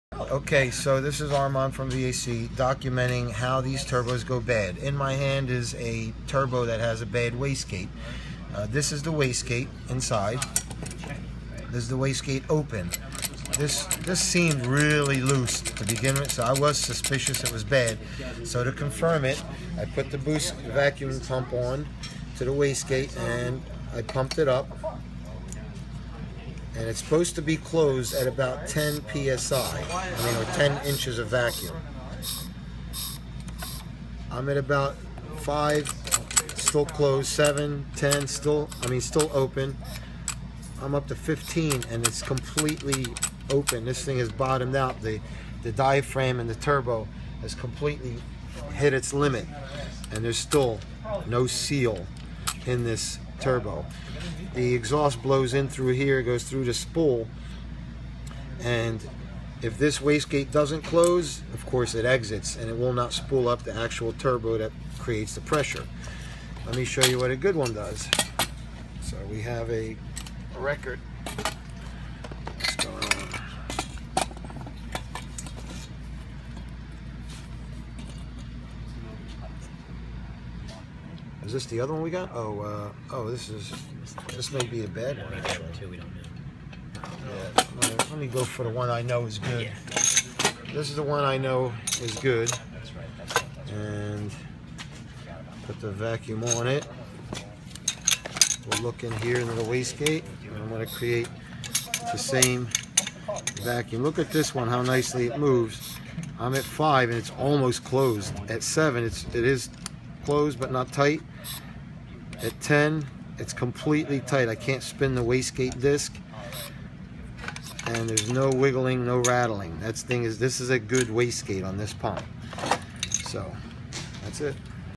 BMW N54 335 535 turbo waste gate 30ff fault test
This gives insight to how a turbo fails and how to test it. When you get a low boost fault 30ff it could be this. Simply removing down pipe and applying vacuum you can confirm.